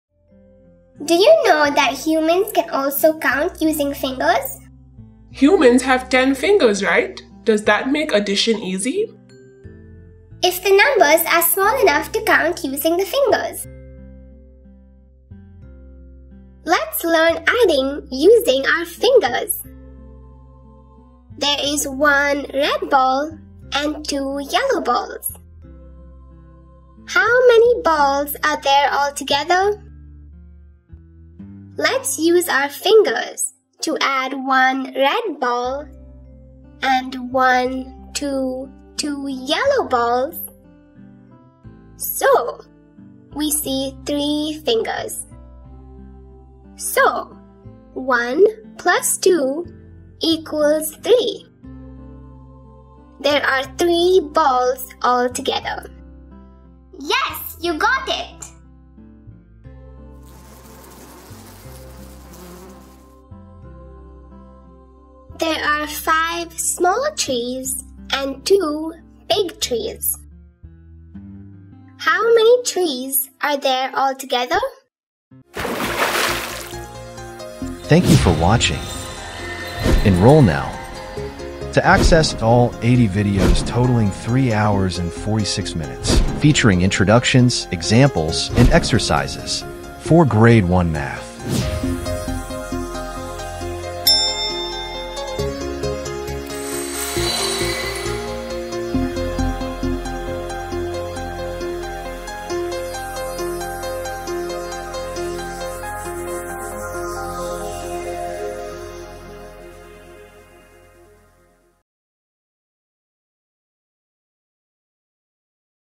G1 - Module 2 - Exercise 4 - Addition Using Fingers | Appu Series | Grade 1 Math Learning
Experience the joy of Addition using Fingers with Appu and Tiger in this hands-on exercise! This fun activity boosts number recognition, strengthens counting concepts, and builds a strong foundation for understanding addition.